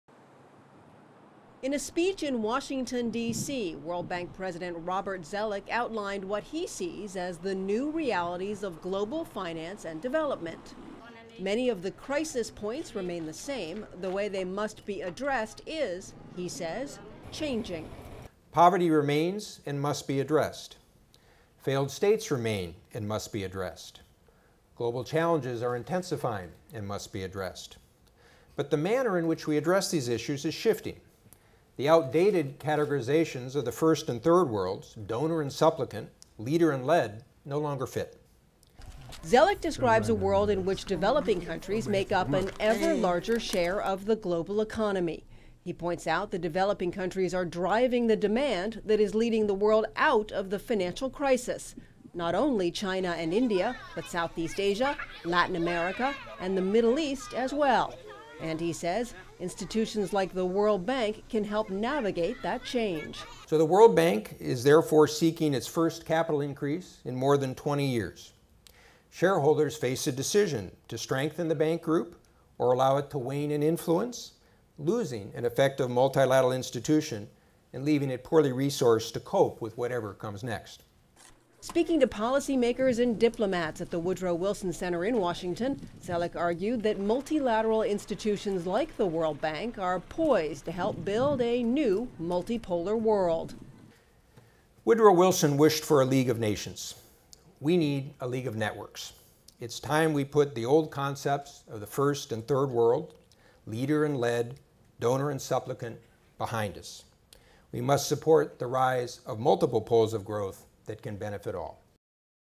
World Bank President Robert Zoellick: The End of the Third World?
Speech by Robert Zoellick, World Bank President, at Woodrow Wilson Center, April 14th, 2010